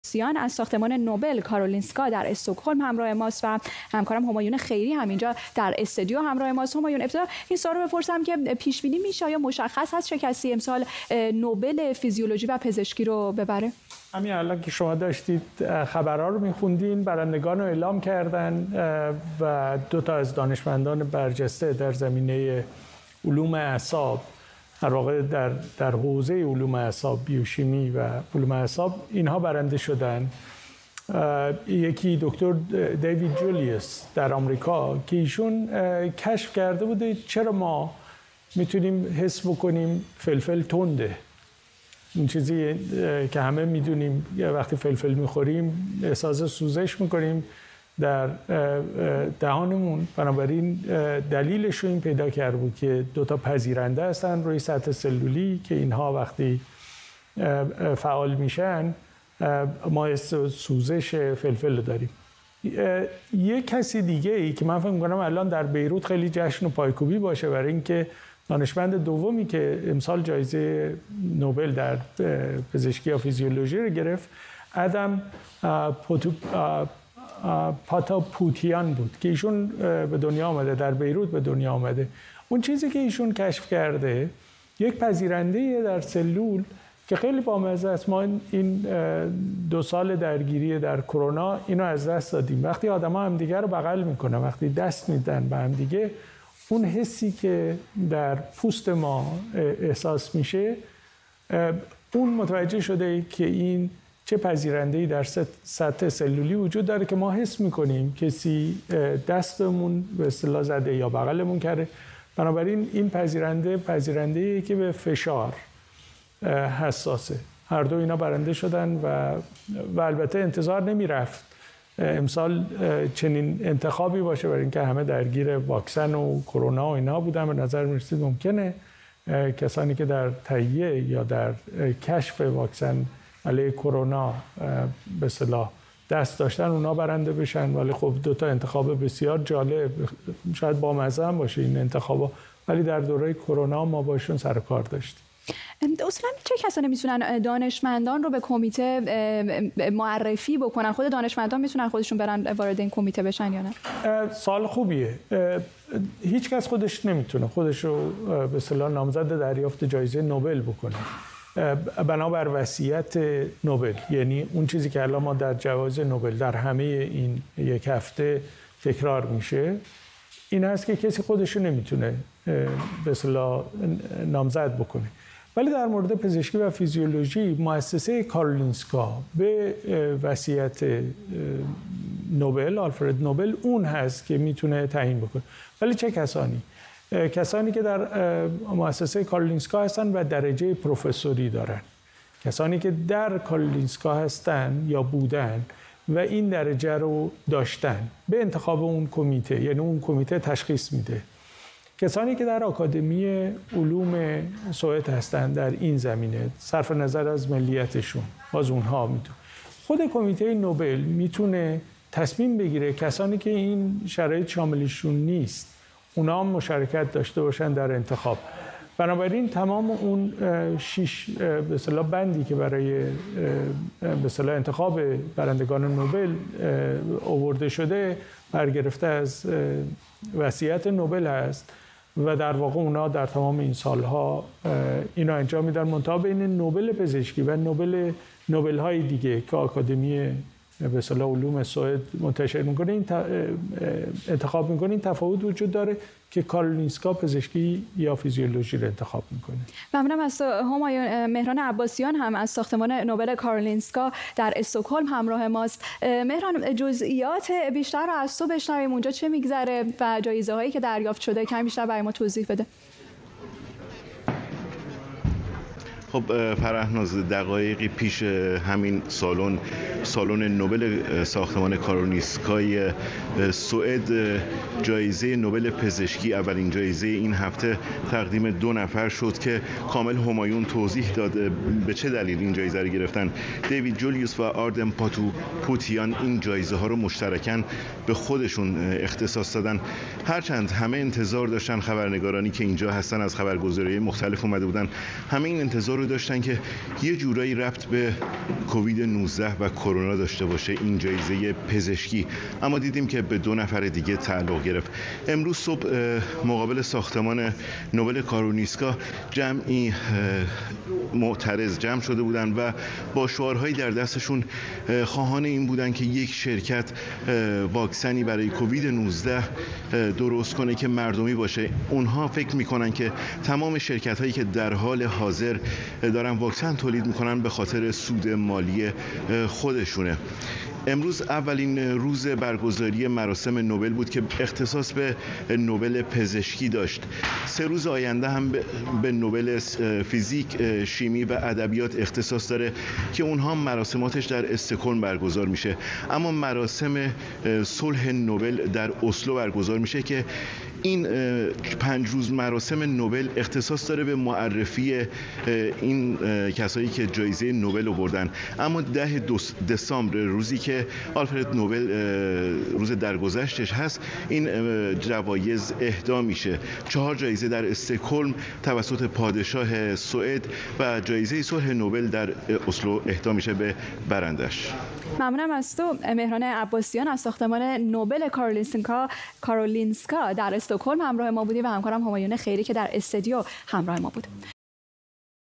برندگان نوبل پزشکی ۲۰۲۱ معرفی شدند
جایزه نوبل پزشکی، به دیوید جولیوس و آردم پاتاپوتیان برای پژوهش در مورد کشف دسته جدیدی از گیرنده های دما و لامسه با استفاده از سلولهای حساس اهدا شد.
گفت‌وگو با همایون خیری و مهران عباسیان، ایران اینترنشنال